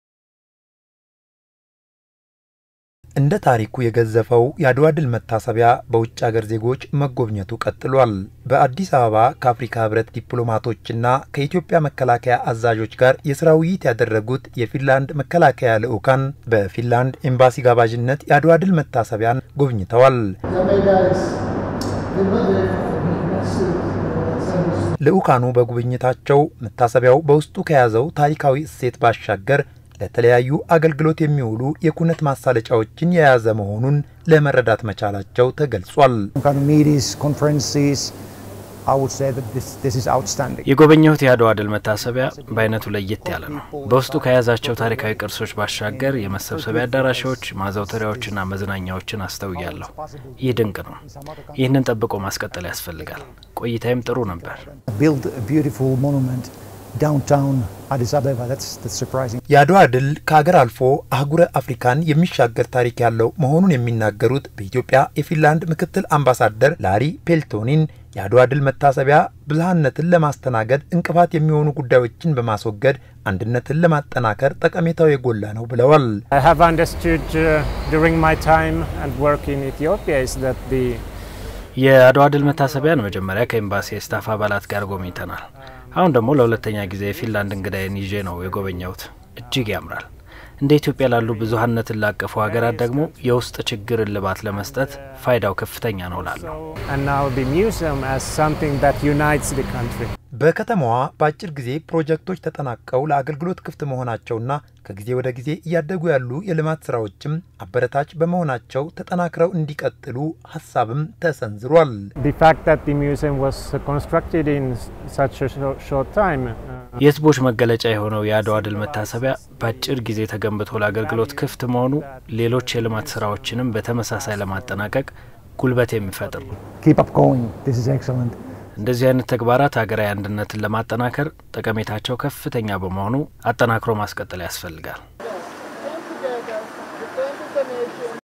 በኢትዮጵያ የፊንላንድ ኤምባሲ ልዑክ የዓድዋ ድል መታሰቢያ ጉብኝት
የዓድዋ ድል መታሰቢያ ብዝኃነትን ለማስተናገድ እንቅፋት የሚሆኑ ጉዳዮችን በማስወገድ ወደ አንድነት ለማምጣት ጠቀሜታው የጎላ ነው ሲሉ በኢትዮጵያ የፊንላንድ ምክትል አምባሳደር ተናገሩ። በኢትዮጵያ የፊንላንድ ኤምባሲ ልዑክ የዓድዋ ድል መታሰቢያን ጎብኝቷል።

AMN - መጋቢት 13/2016 ዓ.ም